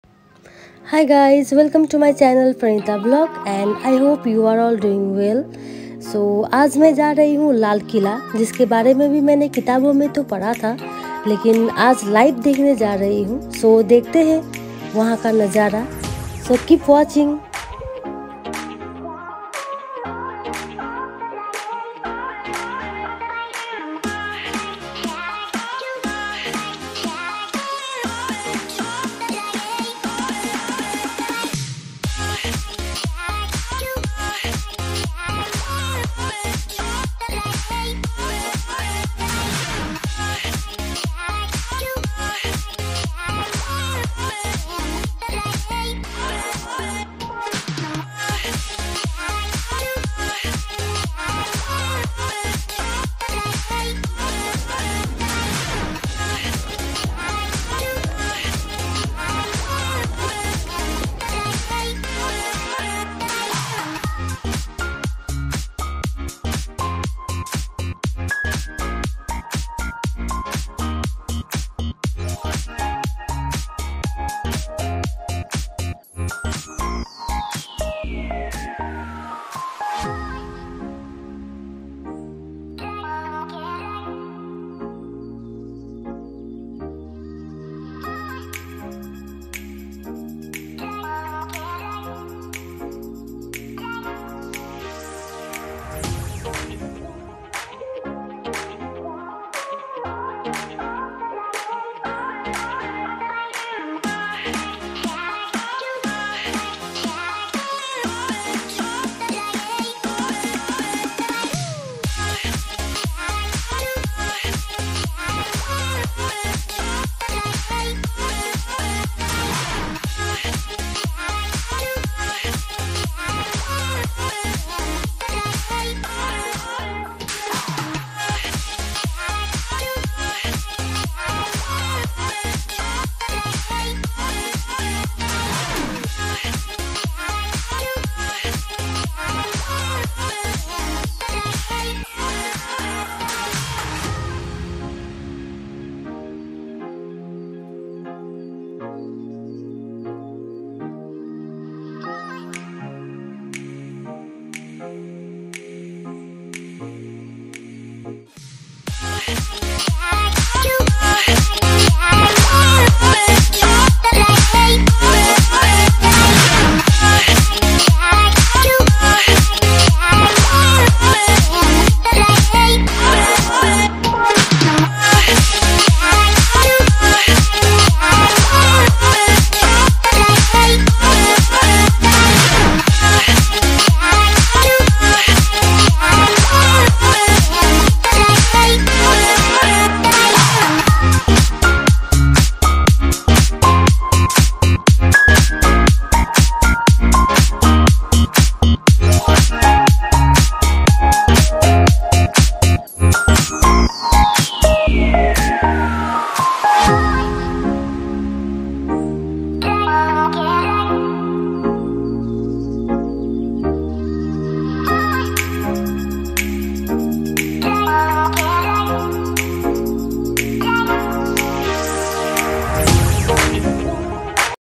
Exploring the Magnificent Red Fort in Delhi | My First Visit | Pranita Vlog
Hey, Vlog fam! 📹✨ In this week's vlog, join me on my very first visit to the iconic Red Fort in Delhi, India. What a breathtaking experience! From the intricate architecture to the rich history, this UNESCO World Heritage Site left me in awe. 🇮🇳❤️

I couldn't help but marvel at the sheer beauty of this historical masterpiece. The Red Fort truly is a symbol of India's glorious past. 🏰✨ Although I couldn't explore the museum this time, I promise to visit it on my next trip to this enchanting place.